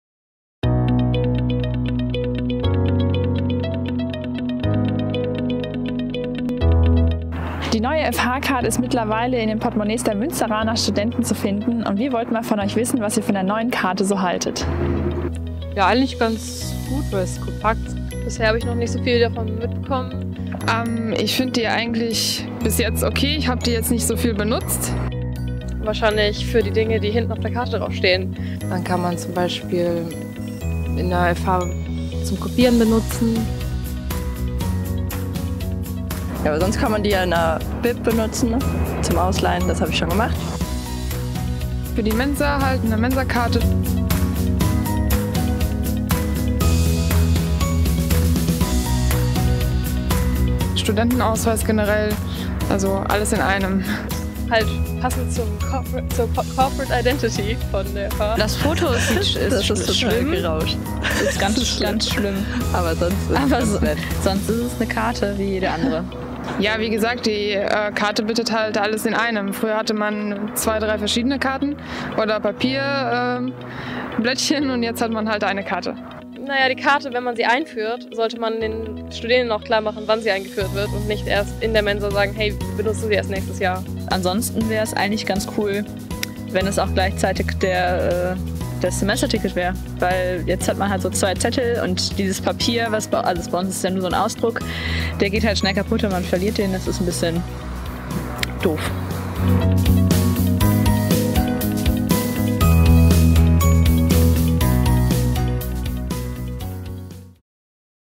SAG MAL 02: Wie findet ihr eigentlich die neue FH-Card?
Seit einigen Wochen gibt es die neue FH-Card. Ein Grund für uns zu fragen, was ihr so davon haltet. Schon mal vorab: auch wir fänden es noch praktischer, wenn auch das Semesterticket auf der Karte integriert wäre, jedoch ist das von anderer Stelle aus verschiedenen Gründen noch nicht möglich. Falls noch Fragen auftauchen, meldet euch gerne bei uns! (Nadine & Kathrin)